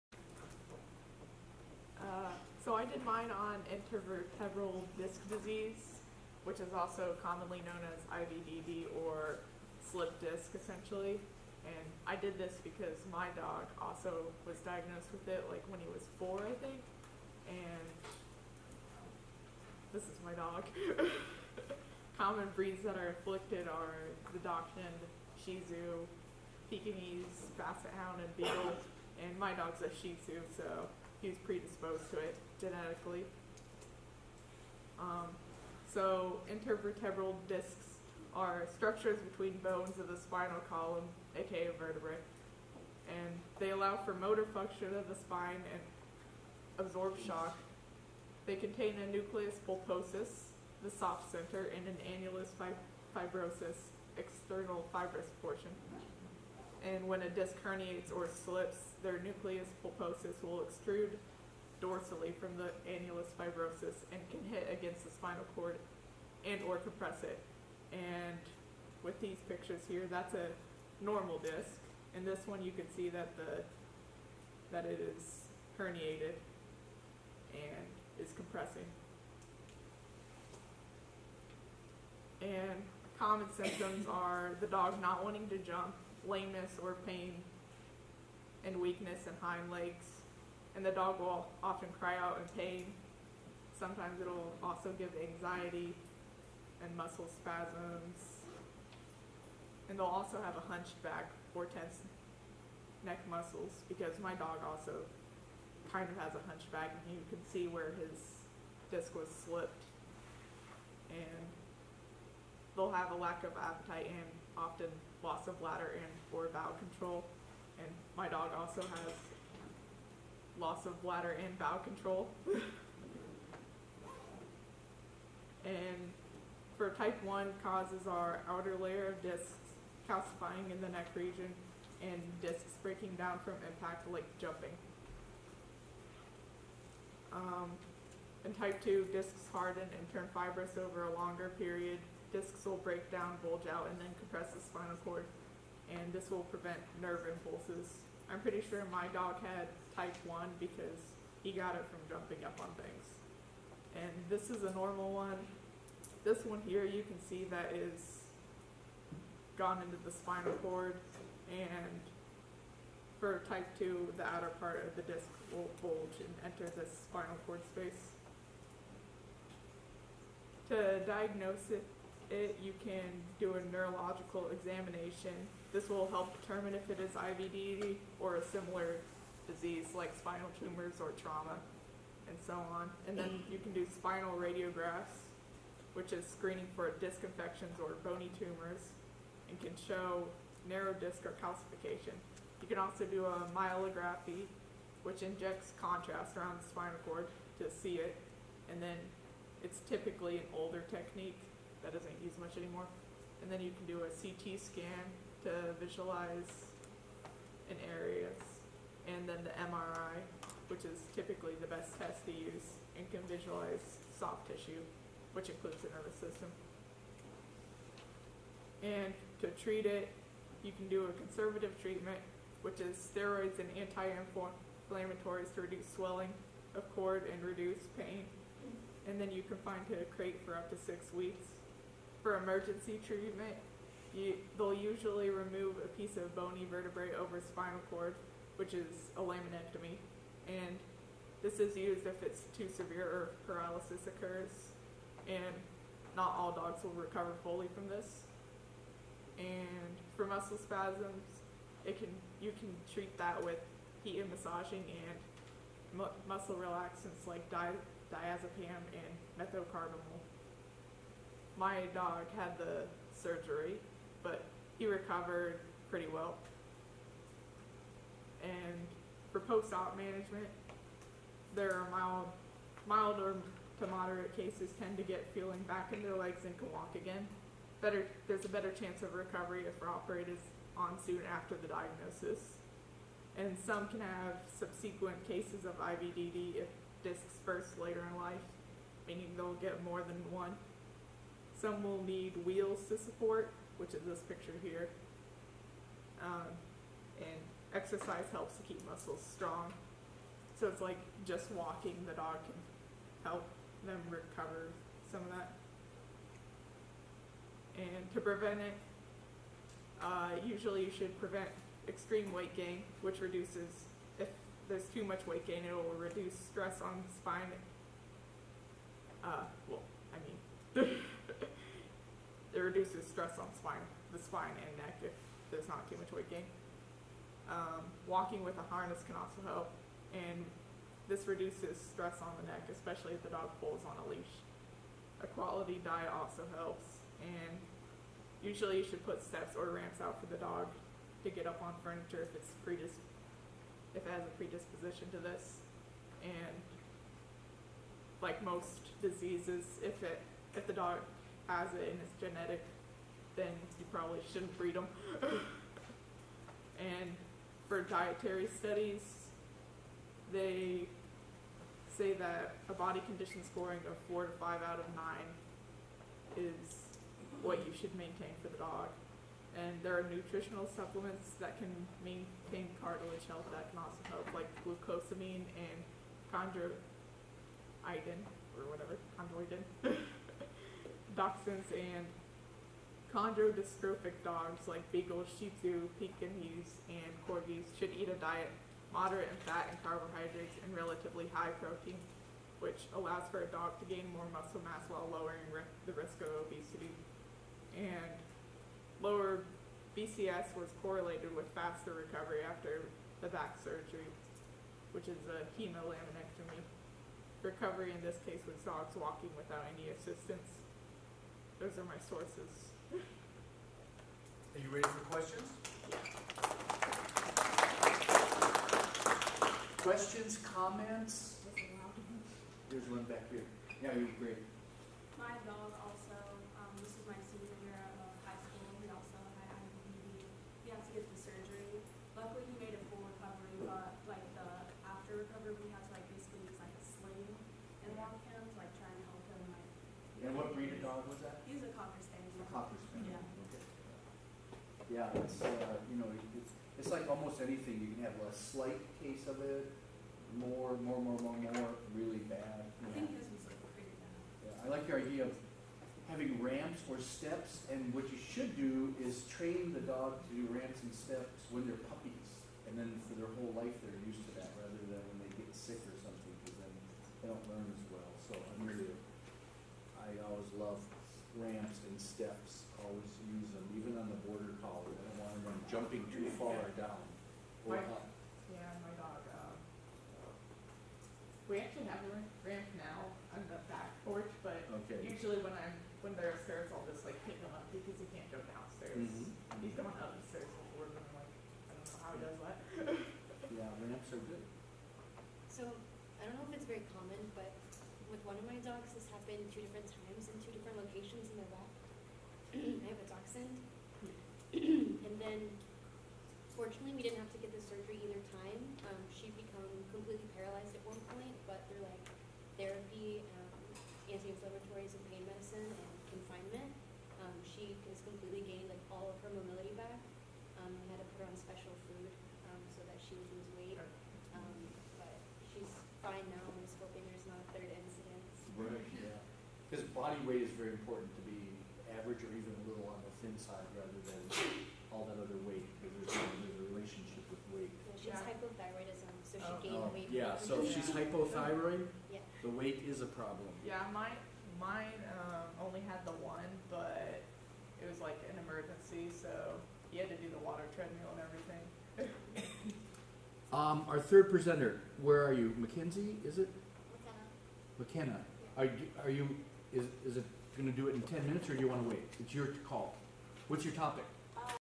Intervertebral Disc Disease (IVDD) in Dogs
A discussion of IVDD in dogs (including comments about a family dog with IVDD).